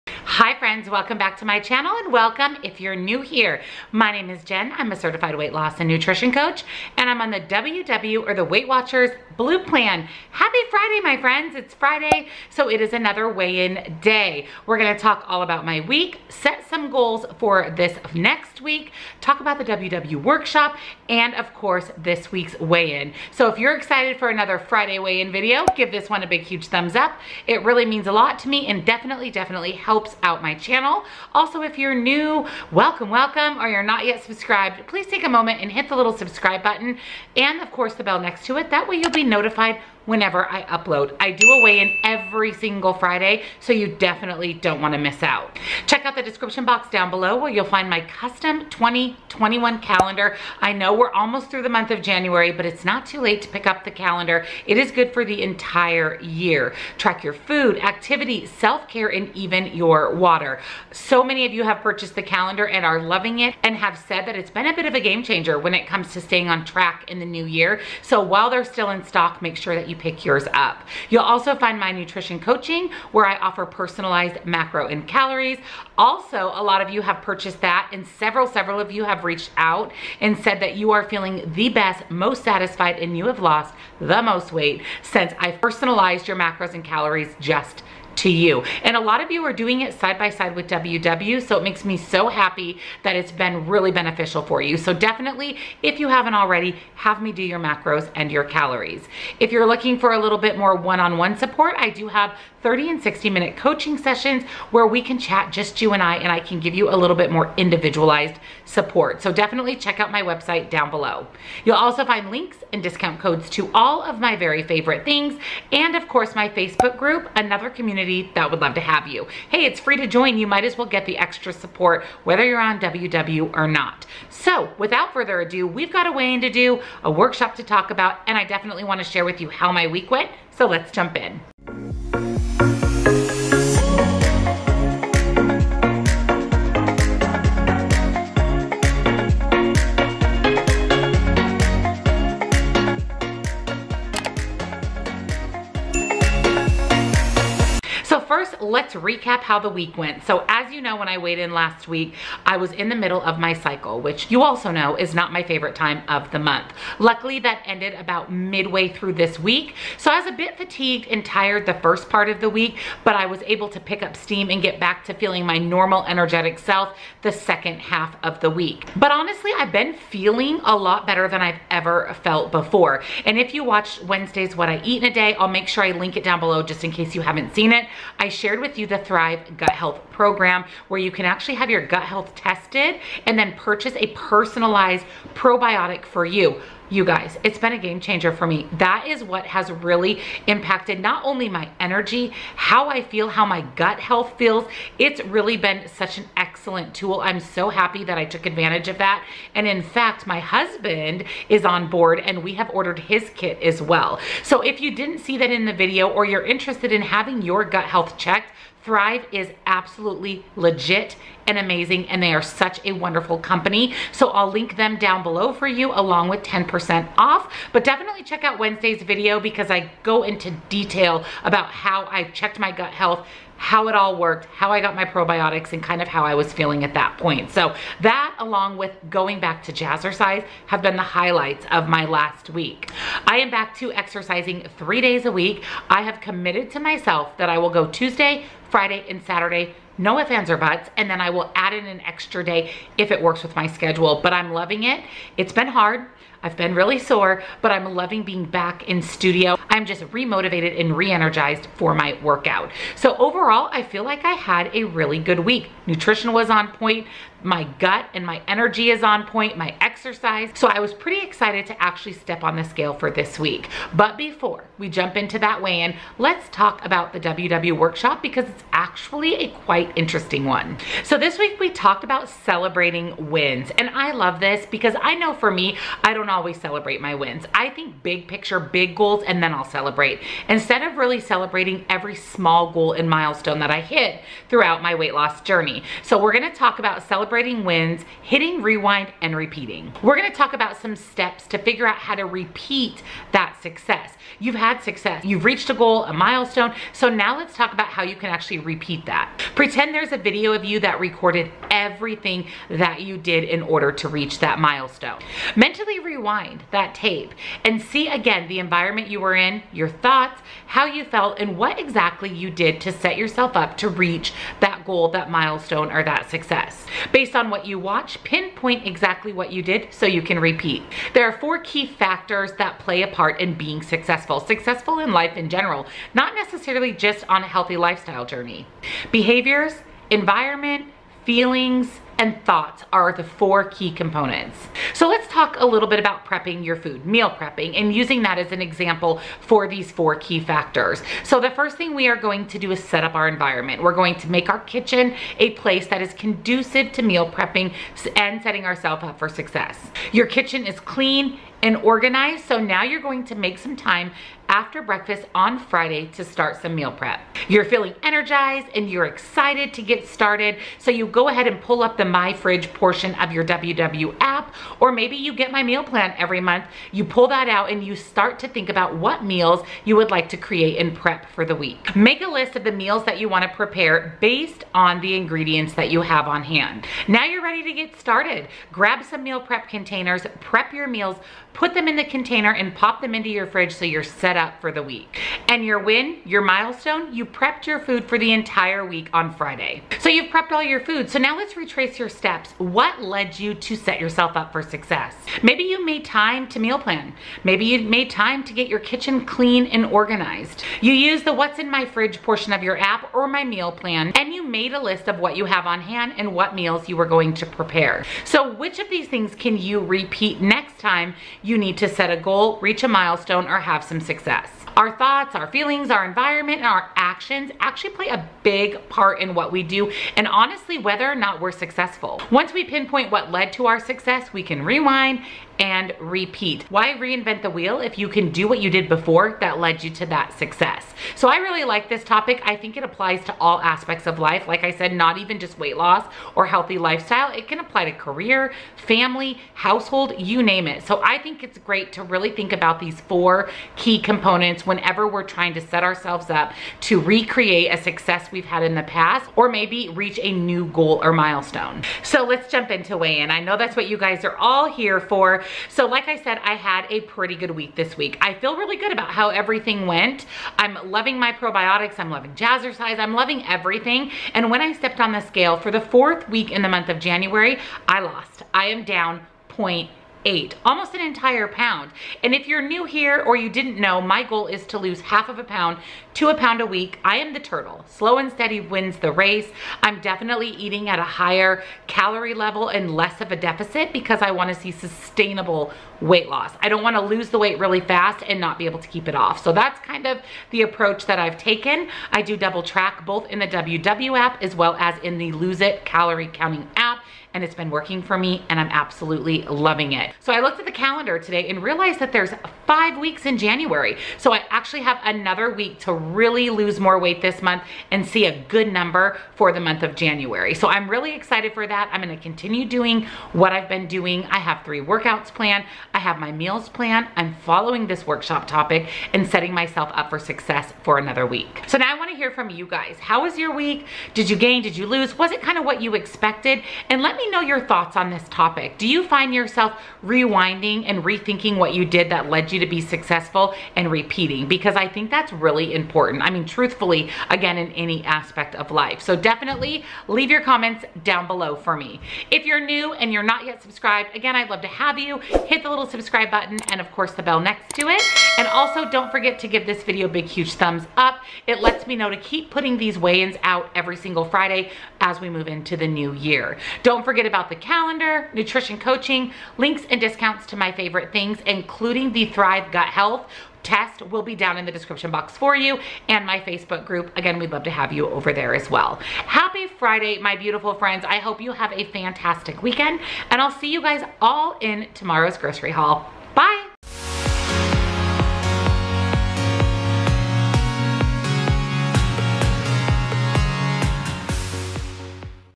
WEEKLY WW WEIGH IN | THIS WEEK...WHAT HAPPENED?? | WEIGHT WATCHERS!!
HAPPY FRIDAY!!  I hope you had a GREAT week!  Today we're talking rewind and repeat and of course I'm sharing this weeks weigh in!  Enjoy!! XO

⬇️ORDER THE CUSTOM 2021 CALENDAR HERE!!

 📙CARRIE ELLE MEAL PLANNER: 📙

Code- jennswwjourney

Code- jennswwjourney

💌HAPPY MAIL!!!💌
Jenn Clayton